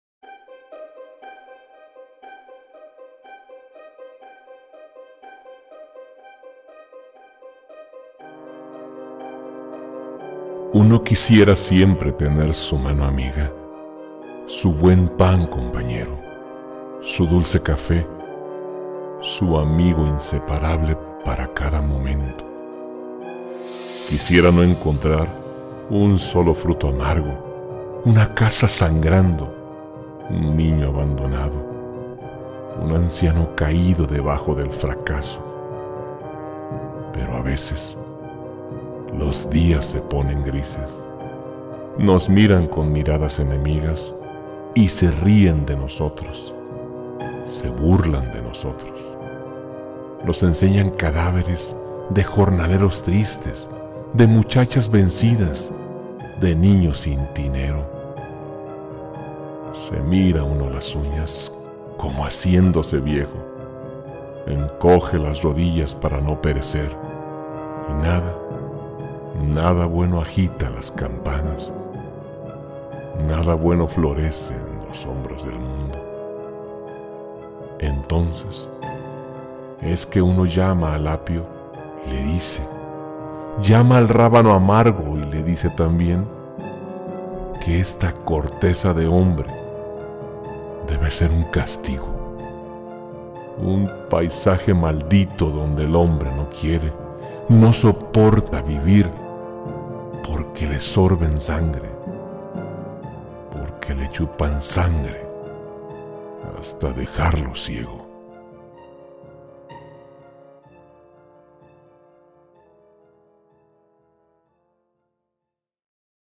ESTE SITIO DE ANGUSTIA - JORGE DEBRAVO
LosTestigosDeMendoza presenta el poema ESTE SITIO DE ANGUSTIA, firmado por el autor Jorge Debravo.

O también puedes escucharnos por
IVOOX:

Uno quisiera siempre tener su mano amiga,
su buen pan compañero, su dulce café, su
amigo inseparable para cada momento.
Quisiera no encontrar un solo fruto amargo,
una casa sangrando, un niño abandonado,
un anciano caído debajo del fracaso.

Pero a veces los días se ponen grises,
nos miran con miradas enemigas,
y se ríen de nosotros,
se burlan de nosotros,
nos enseñan cadáveres de jornaleros tristes,
de muchachas vencidas, de niños sin tinero.
Se mira uno las uñas, como haciéndose viejo,
encoge las rodillas para no perecer,
y nada, nada bueno agita las campanas,
nada bueno florece en los hombros del mundo.

Entonces es que uno llama al apio y le dice,
llama al rábano amargo y le dice también
que esta corteza de hombre debe ser un castigo,
un paisaje maldito donde el hombre no quiere,
no soporta vivir porque le sorben sangre,
porque le chupan sangre hasta dejarlo ciego.

Jorge Debravo